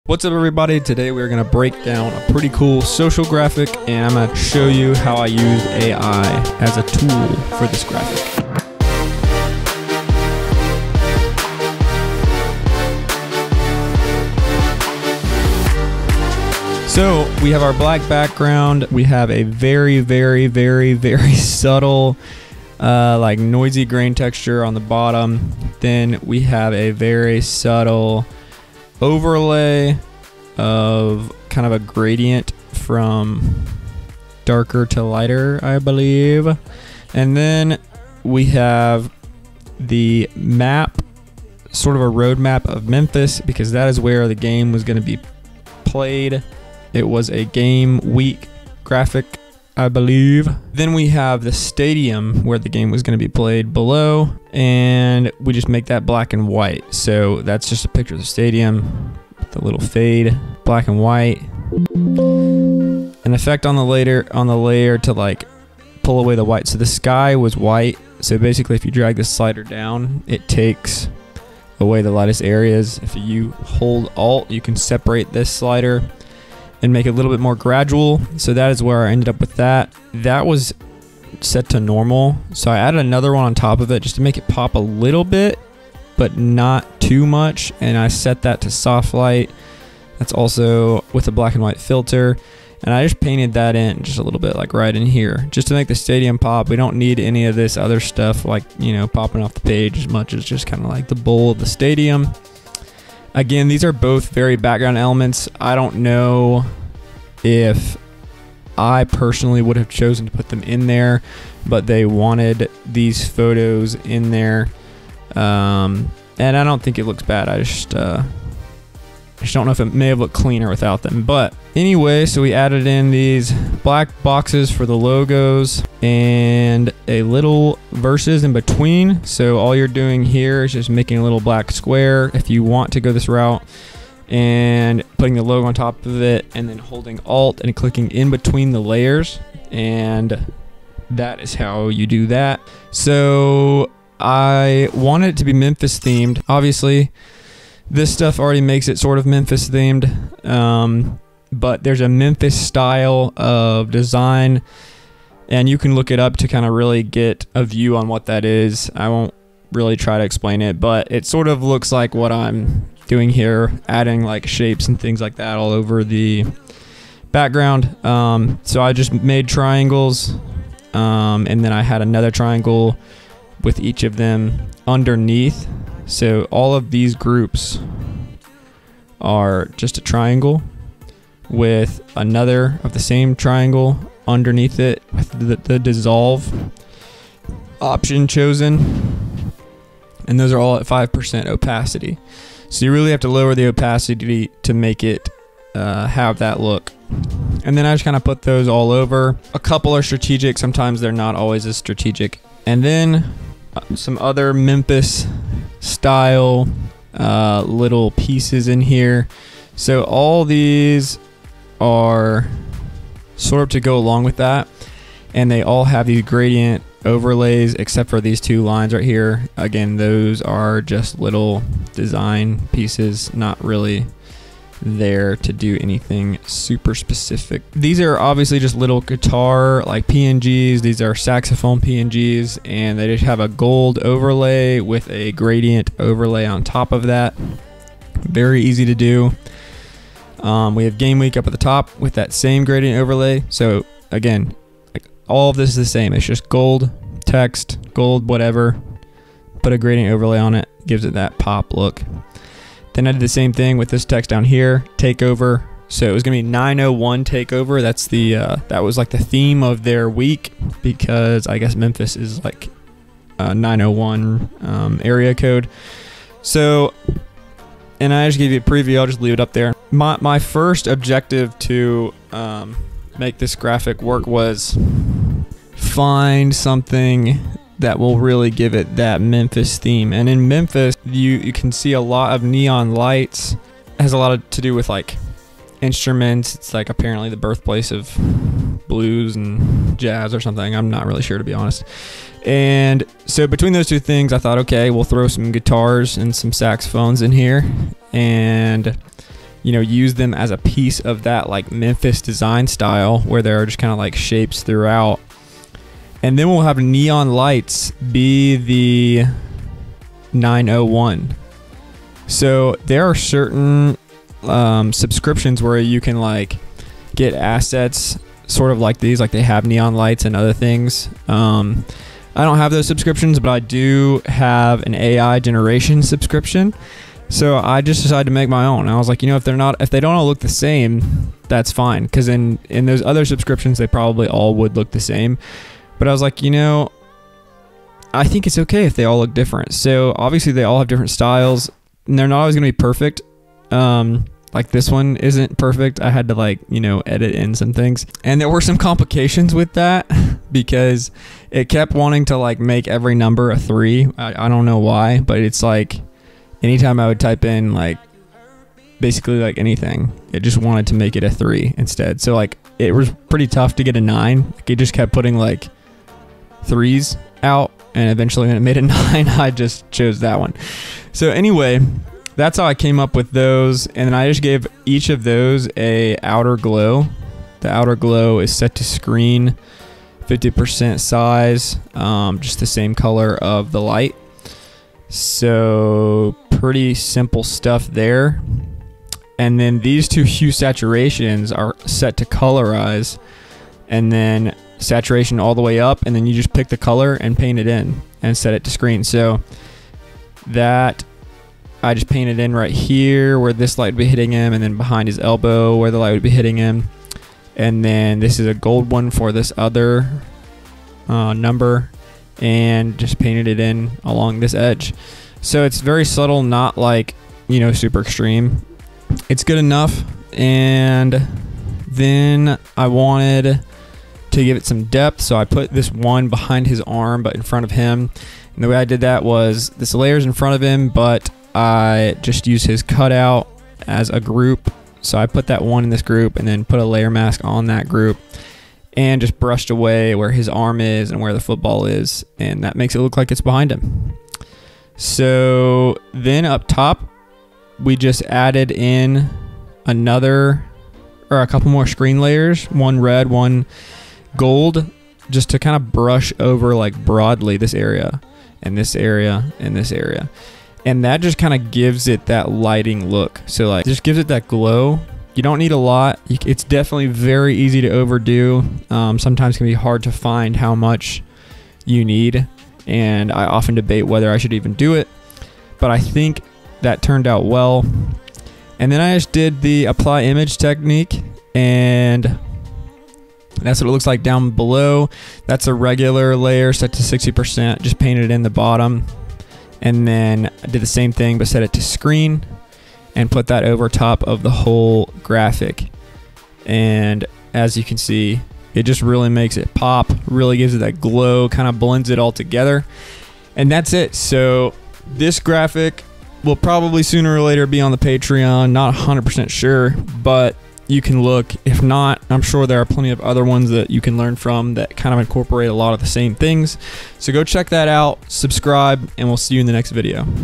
Using AI as a tool for Sports Design
In this video  I break down a sports graphic and show you how I used AI generation as a tool in my graphics.

Where do I get this awesome music?

MY GEAR
Work Computer
Cam Mic
24-70 Lens
70-180 Lens
Data Storage
RGB Lights
Camera Grip